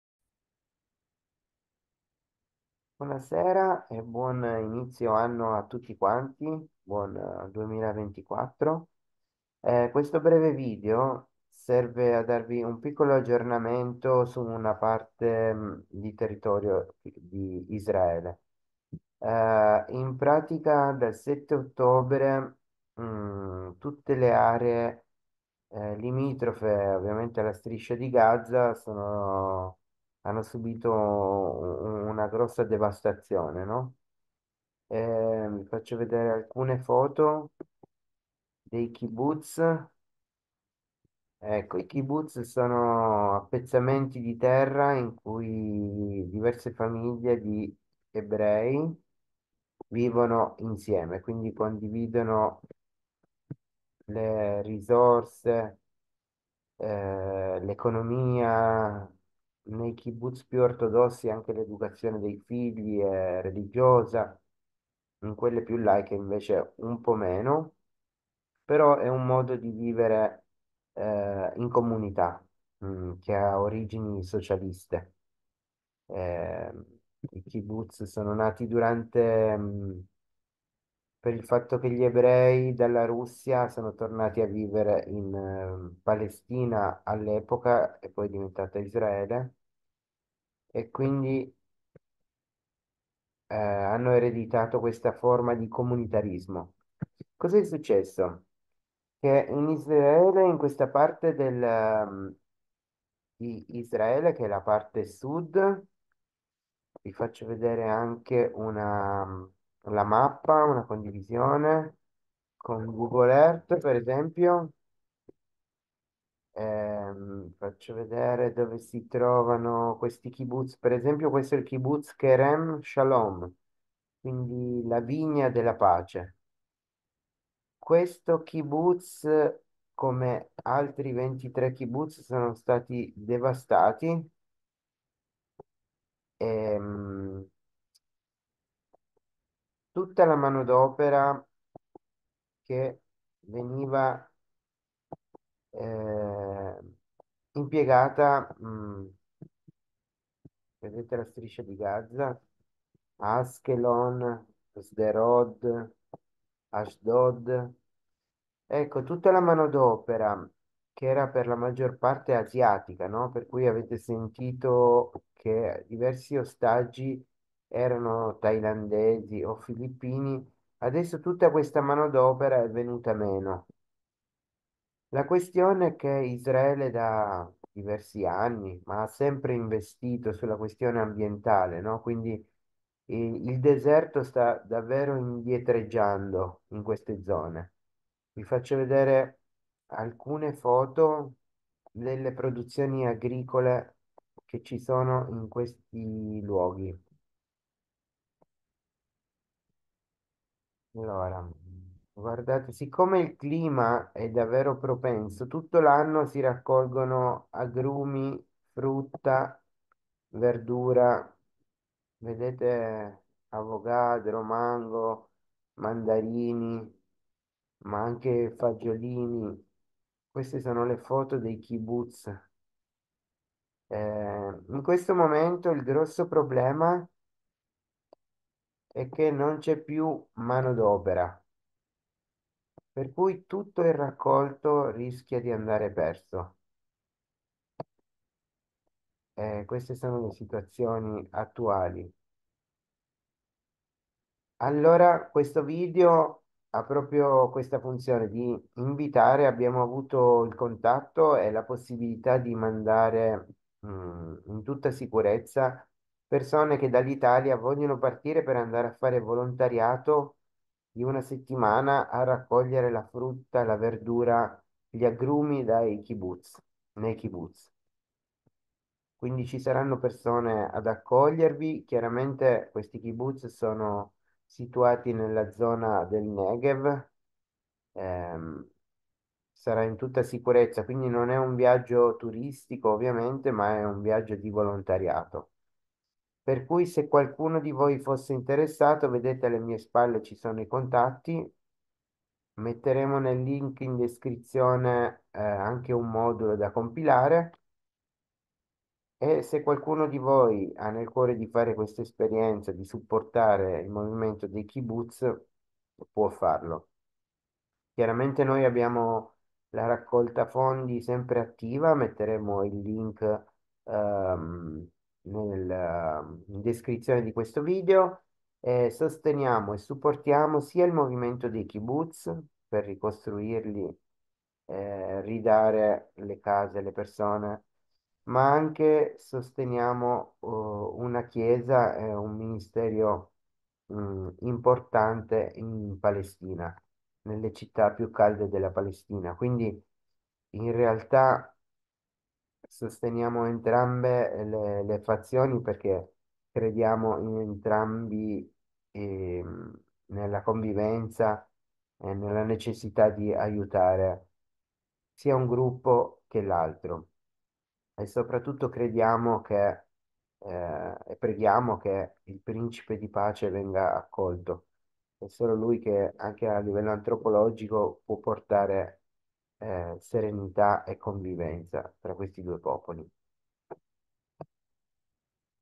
Cercasi Volontari nei kibbutz in Israele
Continua sostenere la raccolta fondi per Israele

Fai volontariato in Israele per la raccolta della frutta